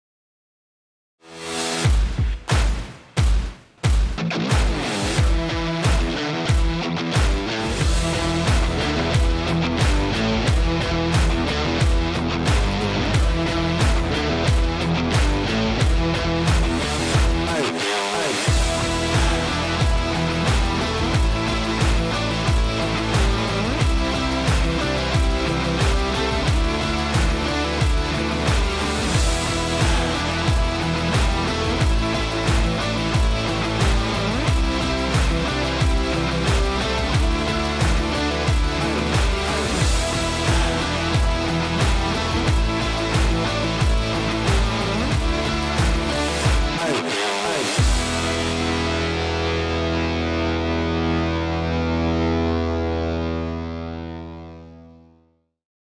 How to solve Hitachi air conditioner daily work of HVAC professionals issues: The Solvers are back!
The Solvers come back, the series of tutorial videos for HVAC technicians produced by Hitachi Cooling & Heating to help solve specific day-to-day work of professionals problems on Hitachi air conditioners.

In each episode, we take a look at the situation, its possible causes or step to follow up, and the action that needs to be taken. Let's go!

Have The Solvers helped you fix and resolve your doubts?